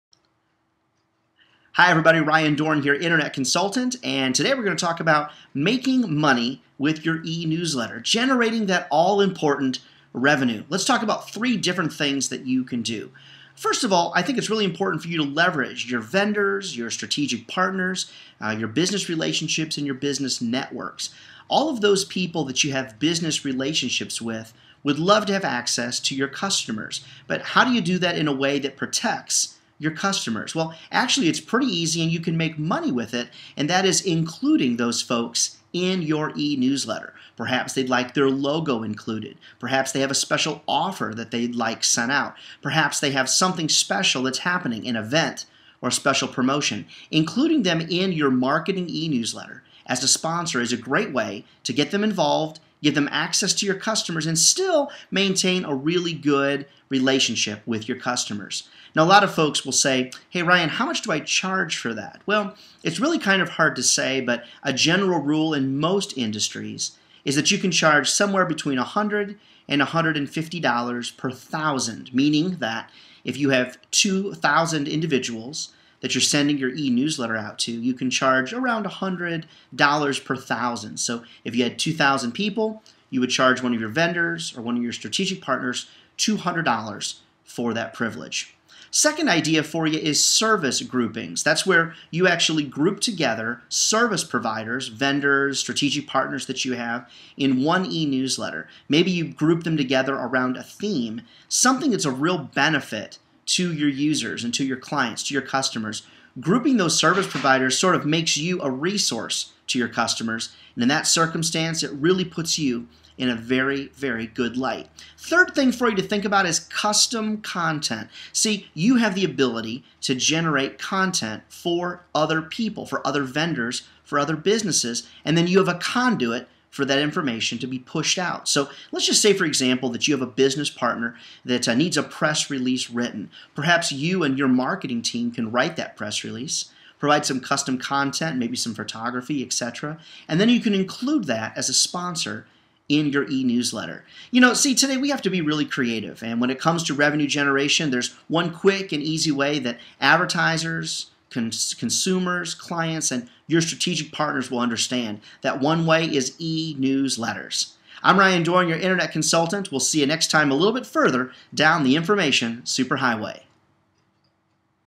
eNewsletter Revenue Tips
Internet consultant Ryan Dohrn shares 3 tips to help business owners generate revenue from their marketing eNewsletters.